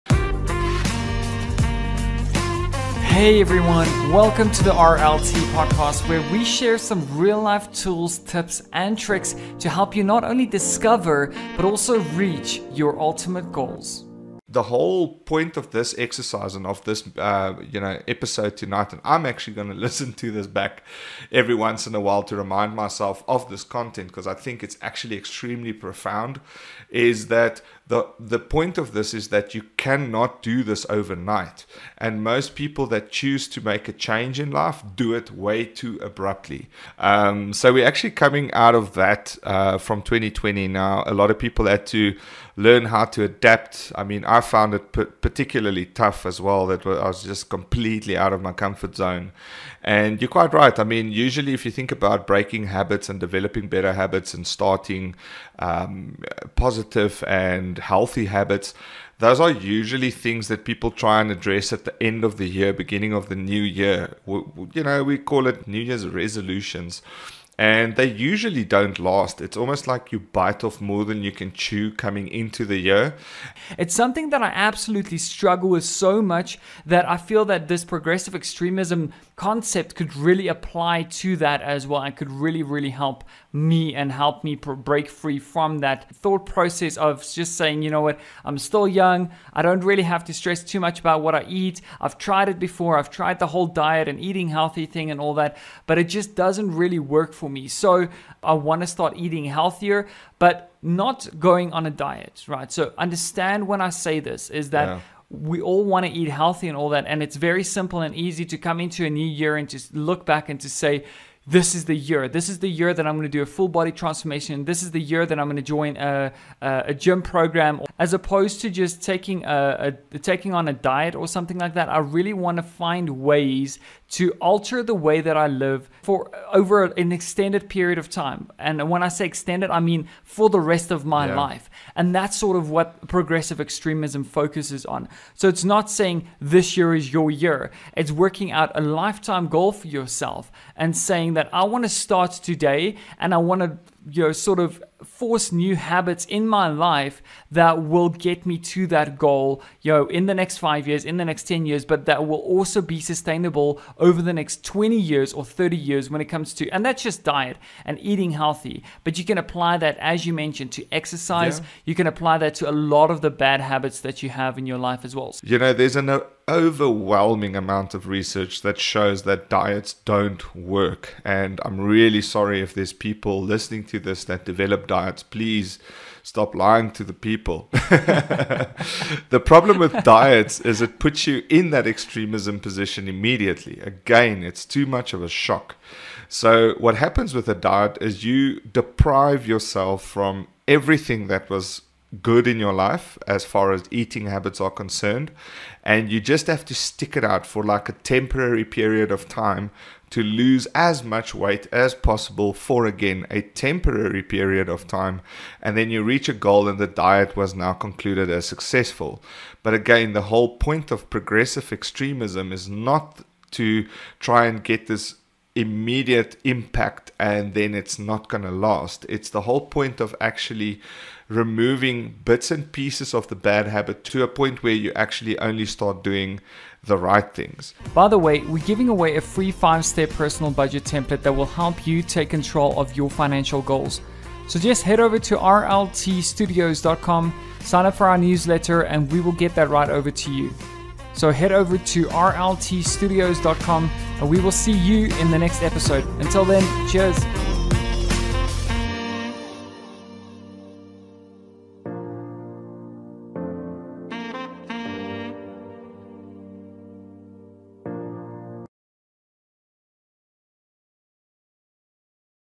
016 | Breaking bad habits: Progressive extremism (Preview)
It's simple to say "man, just stop eating unhealthy" or "I should really stop watching so much TV during the week", but we all know it's not that easy to break bad habits. In episode 16 tomorrow (19 Apr 2021) we share a great method of "chopping down the tree; branch by branch" and form healthier, more sustainable long term habits. Here's a sneak preview!